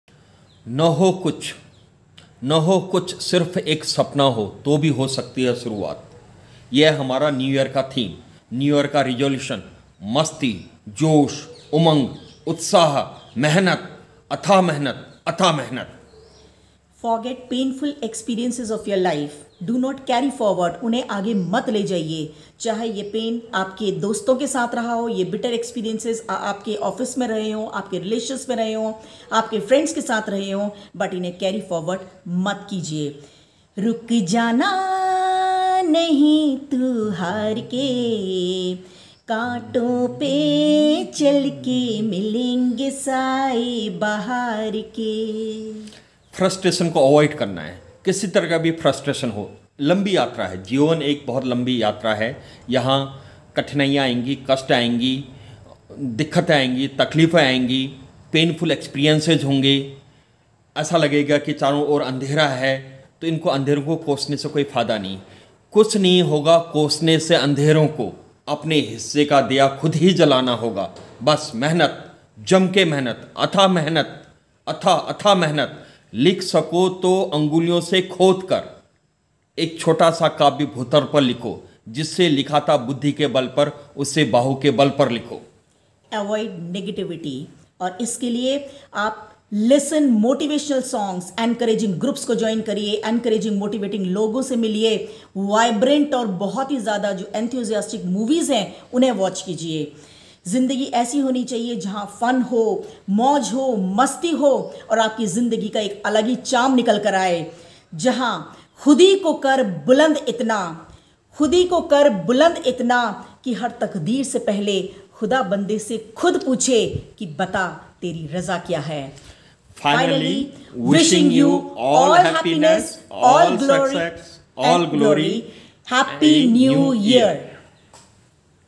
New Year Resolution and Theme
Even if you have nothing, but have the dreams. you can give a new start to your life. These inspirational lines from Venugopal is my new year resolution and theme.
The whole theme of new year resolution is summer up in the following points:
1. There must be dreams...there must be hope and optimism.
2. Make all the pains of the past a forgotten episode and look for better, brighter and beautiful future ahead.
3. No need to be frustrated with the things around, you just keep on doing your bit.
4. Be brave & work hard. this is the only panacea to overcome your sufferings.
5. Ward off negativity at every point and at every cost. Replace the negative conditions & situations with positive emotions.
6. Celebrate relations, life and live fullest.